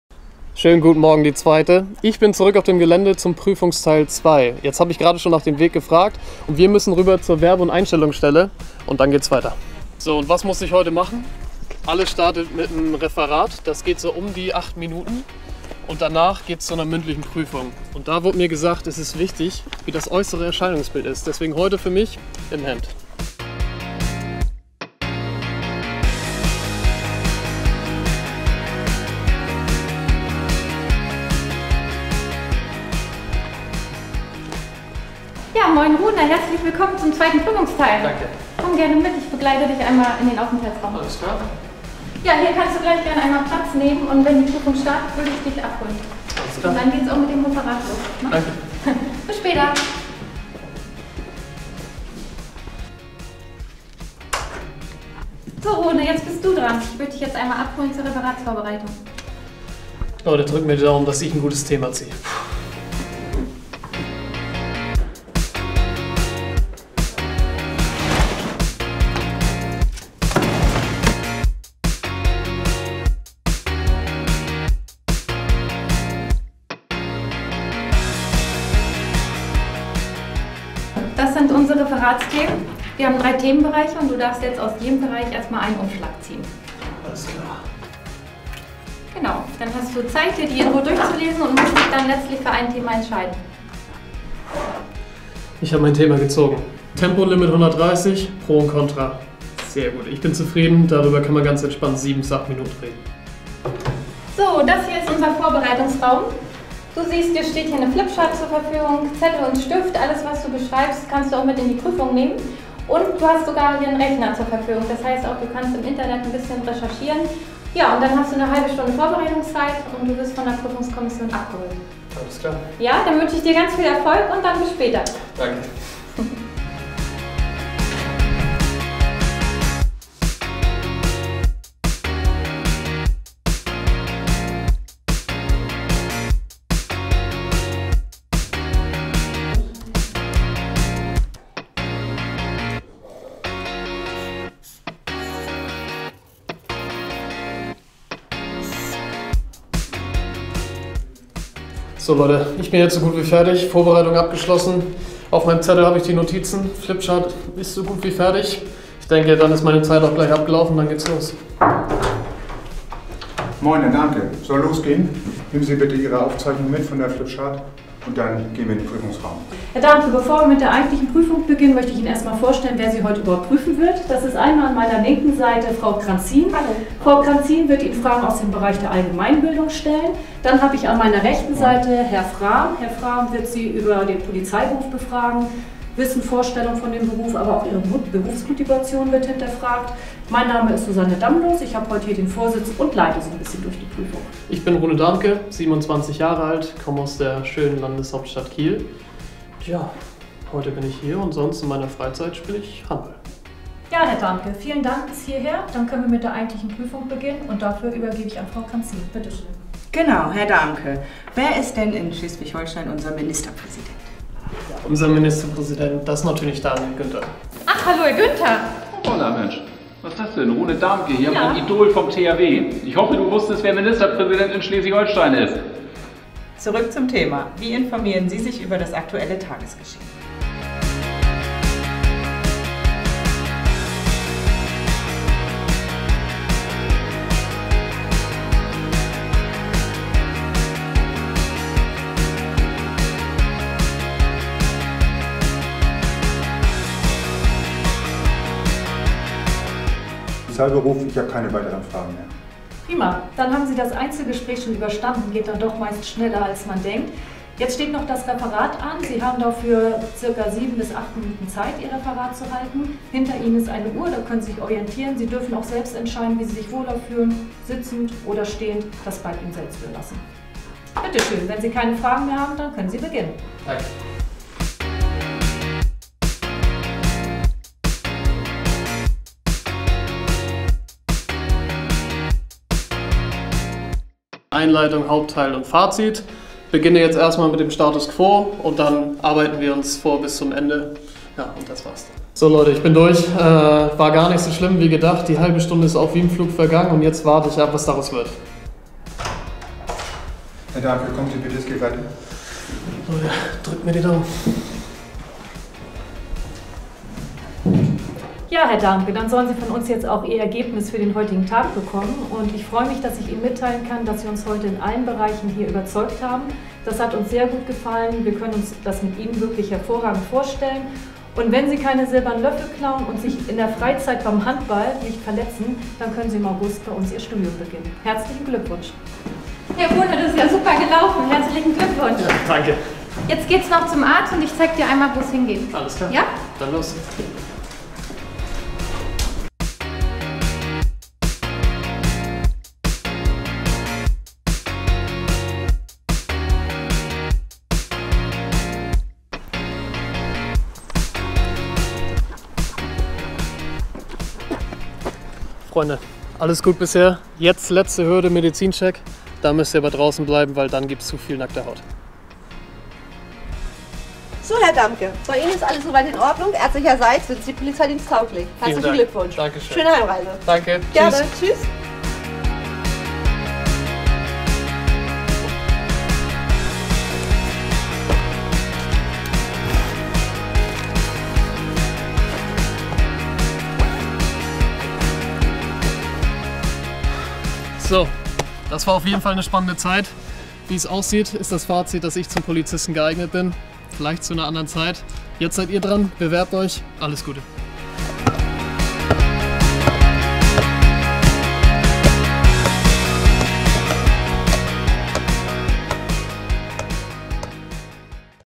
Einstellungstest: "Anwurf in blau Teil 2"
Profihandballer Rune Dahmke: eben noch beim THW Kiel - jetzt in unserem Testverfahren zum Polizisten.  Hier stellt er sich dem zweiten Prüfungstag.